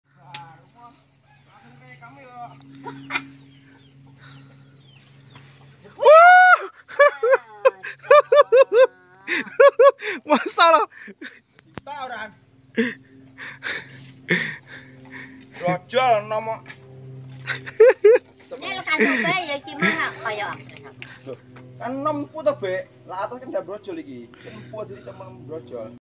Sumberjambe, wonorejo, Temurejo || Banyuwangi selatan diserbu hama belalang
Di Banyuwangi lagi musim belalang